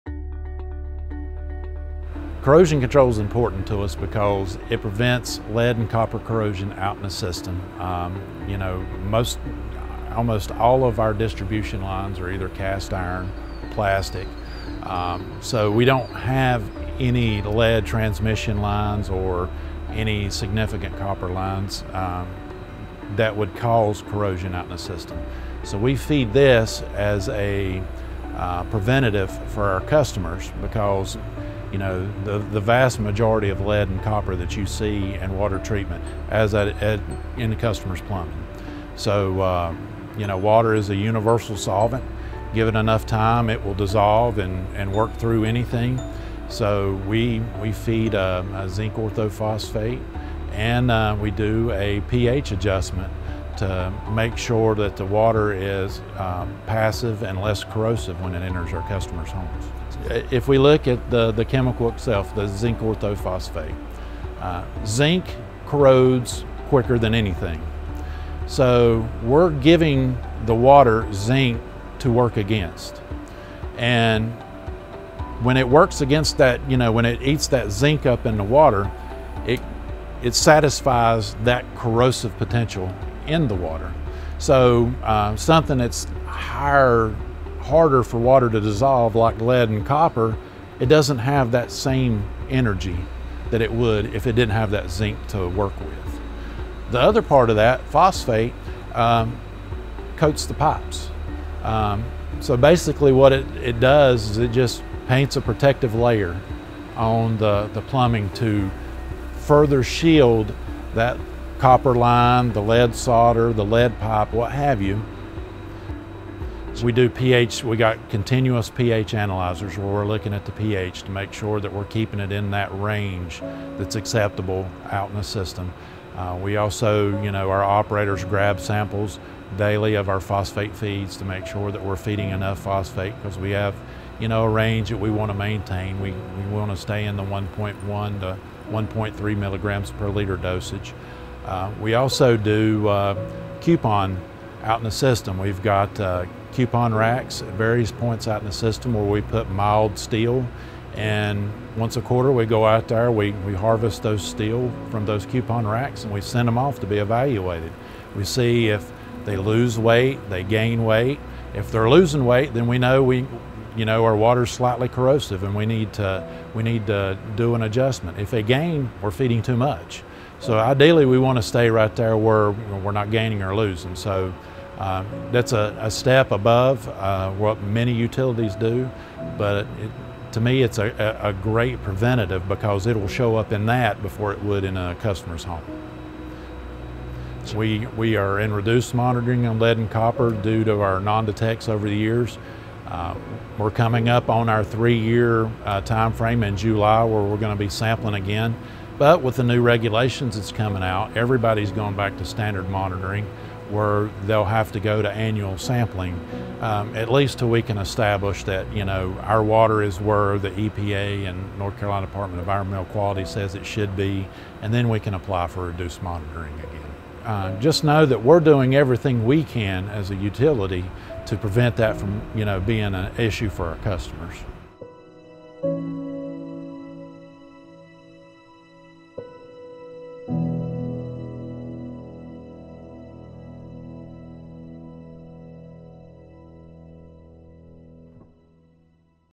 PWC and Corrosion Control Program
Learn more about PWC's Corrosion Control Program that prevents metals like lead and copper from leaching from a homeowners plumbing into their drinking water.